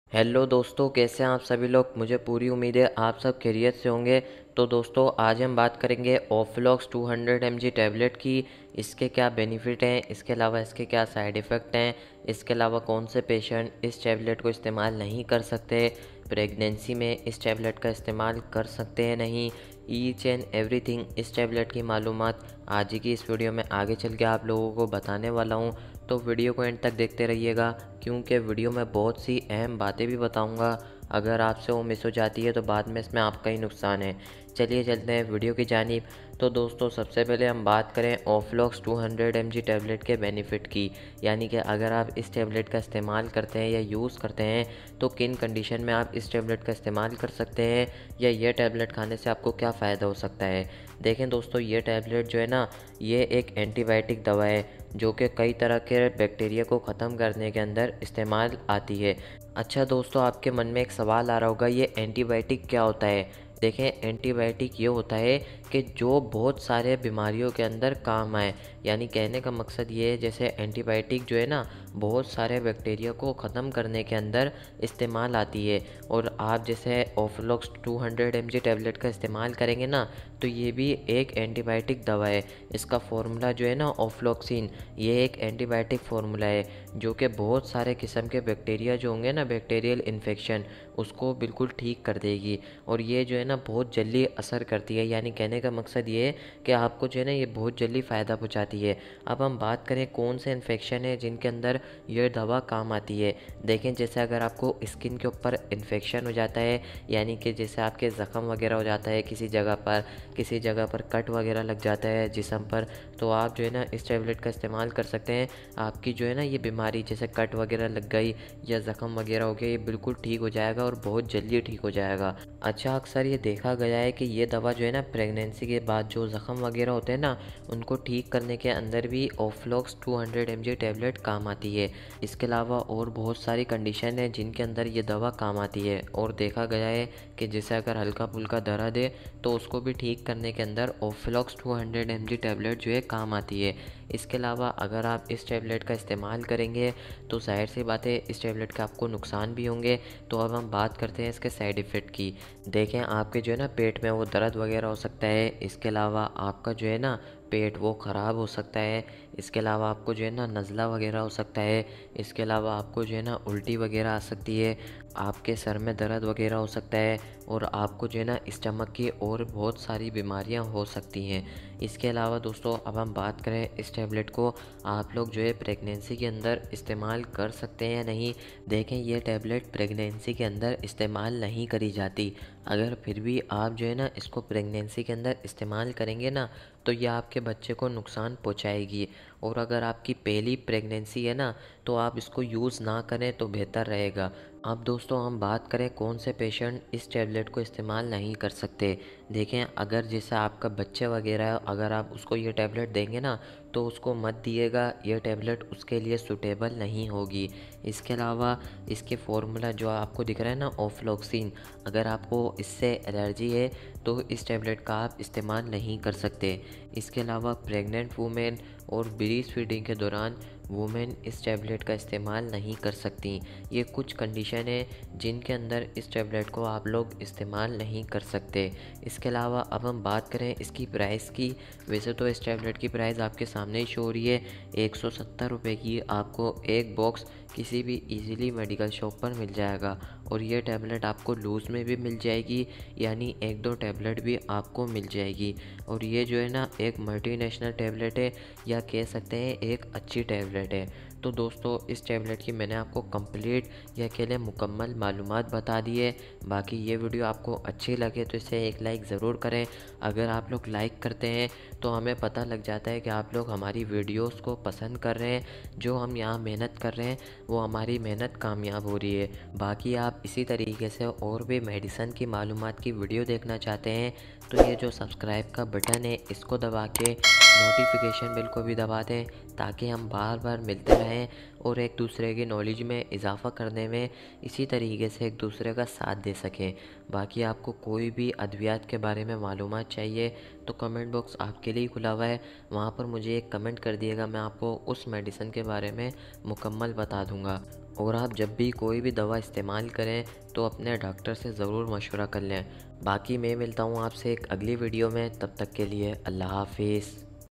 OFLOX 200mg Tablet Uses in Urdu | Best Antibiotic Tablet For Anti Bacterial Medicine
OFLOX 200mg Tablet Uses in Urdu

Topic Cover !!!
OFLOX 200mg Tablet Benefits
OFLOX 200mg Tablet Side Effect
OFLOX 200mg Tablet Pregnency Safe or Not
OFLOX 200mg Tablet Kon sa Patient Use Nahi Kar Sakte
OFLOX 200mg Tablet Dosage

Is video mein apko OFLOX 200mg tablet ka bare mein mukammal batoa ga iska benefits batoa ga iska side effect batoa ga iski dosage batoa ga iski bare mein pregnency safe ha ya nahi ya bhi batoa ga iska ilawa kon sa patient use nahi kar sakte ya bhi batoa ga or iska ilawa bhi bhut kuch batoa ga janna ka liya video watch kare OR HAA VIDEO LIKE KAR DENA!

Meri Baki Branches !!!